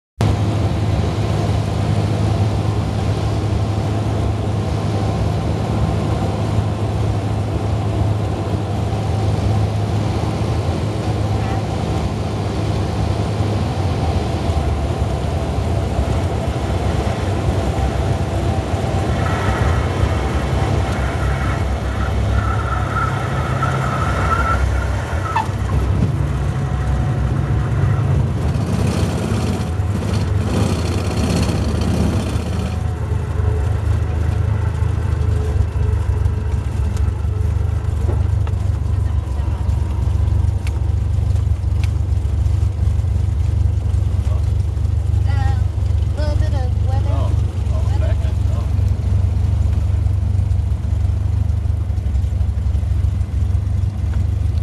windy landing with Dad, SF Bay 5-15-10
heavy northwest wind, flight instructor Sofia.